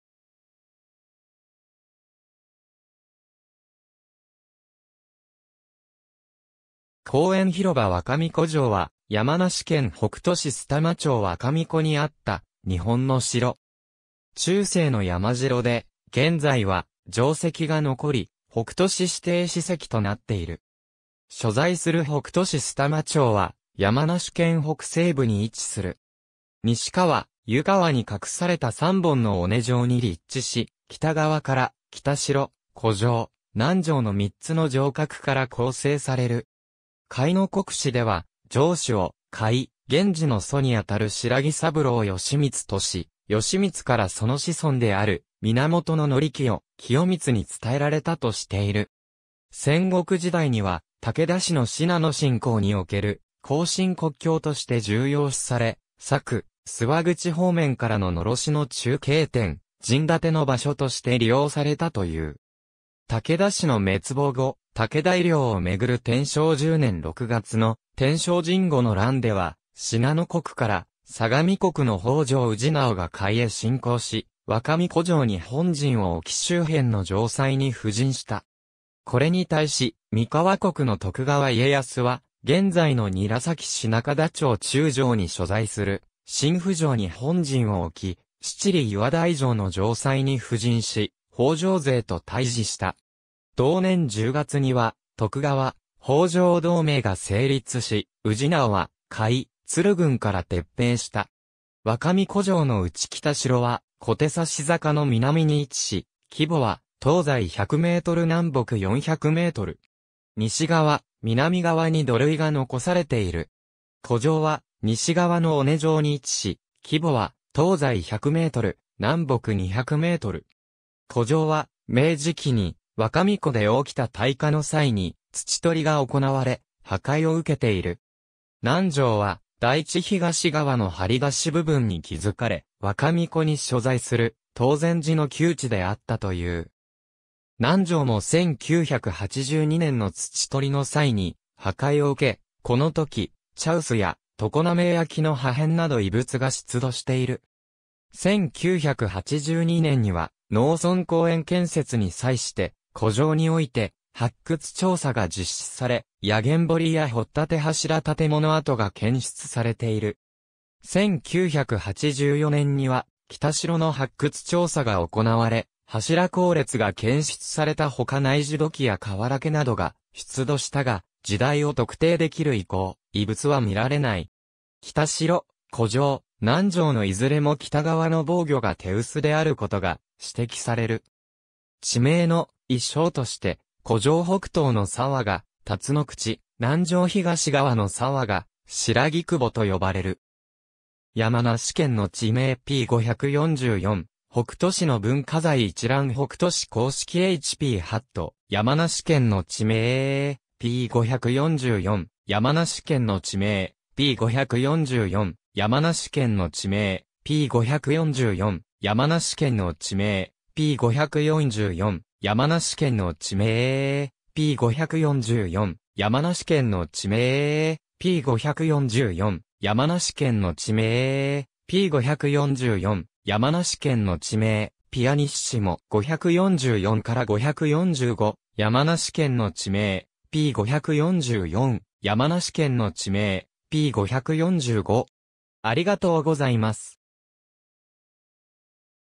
若神子城
公園広場 若神子城（わかみこじょう）は山梨県北杜市須玉町若神子にあった日本の城。
中世の山城で、現在は城跡が残り（若神子城跡）、北杜市指定史跡となっている。
所在する北杜市須玉町は山梨県北西部に位置する。
西川・湯川に画された三本の尾根上に立地し、北側から北城・古城・南城の三つの城郭から構成される。
『甲斐国志』では、城主を甲斐源氏の祖にあたる新羅三郎義光とし、義光からその子孫である源義清・清光に伝えられたとしている。
戦国時代には武田氏の信濃侵攻における甲信国境として重要視され、佐久・諏訪口方面からの狼煙の中継点、陣立ての場所として利用されたという。
武田氏の滅亡後、武田遺領をめぐる天正10年（1582年）6月の天正壬午の乱では、信濃国から相模国の北条氏直が甲斐へ侵攻し、若神子城に本陣を起き周辺の城砦に布陣した。
これに対し、三河国の徳川家康は現在の韮崎市中田町中條に所在する新府城に本陣を起き、七里岩台上の城砦に布陣し、北条勢と対峙した。
同年10月には徳川・北条同盟が成立し、氏直は甲斐・都留郡から撤兵した。
若神子城のうち北城は小手指坂の南に位置し、規模は東西100メートル南北400メートル。
西側・南側に土塁が残されている。
古城は西側の尾根上に位置し、規模は東西100メートル、南北200メートル。
古城は明治期に若神子で起きた大火の際に土取が行われ、破壊を受けている。
南城は台地東側の張り出し部分に築かれ、若神子に所在する東漸寺の旧地であったという。
南城も1982年（昭和57年）の土取の際に破壊を受け、このとき茶臼や常滑焼の破片など遺物が出土している。
1982年（昭和57年）には農村公園建設に際して、古城において発掘調査が実施され、薬研堀や掘立柱建物跡が検出されている。
1984年（昭和59年）には北城の発掘調査が行われ、柱坑列が検出されたほか内耳土器やかわらけなどが出土したが、時代を特定できる遺構・遺物は見られない。
北城・古城・南城のいずれも北側の防御が手薄であることが指摘される。
地名の遺称として、古城北東の沢が「たつのくち」、南城東側の沢が「新羅くぼ」と呼ばれる。